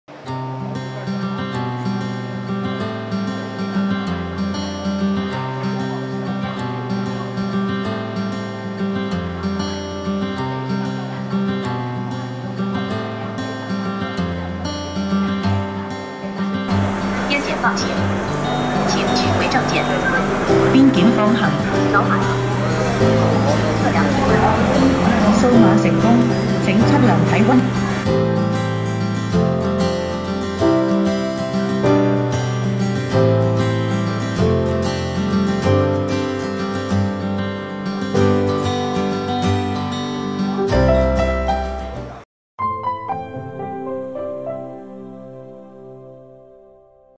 “港车北上”政策正式实施 首辆香港单牌车驶入广东
7月1日零时，“港车北上”政策正式实施。当日凌晨，一辆车牌号为LL7999的香港机动车通过查验后驶入珠海，成为首辆进入广东的香港单牌车。
记者现场看到，伴随着一声“系统放行，请通行”的指引，首车车辆经过车体检查后，借助“一站式”车辆通关系统自动读取车辆和司机证件、指纹、人脸信息等。十几秒的等待之后，便顺利通行。
此次“港车北上”，香港车主只要年满18周岁、持有港澳居民来往内地通行证，本人名下登记持有1辆非营运小型载客汽车，就可以申请，没有配额限制，没有投资纳税及特定身份等条件限制。香港车主可登录“港车北上”信息管理服务系统申请，不用出香港即可一网申办。
据介绍，目前，香港已有超过1.64万人登记申请，已完成三轮“港车北上”抽签，约7700人中签可递交申请。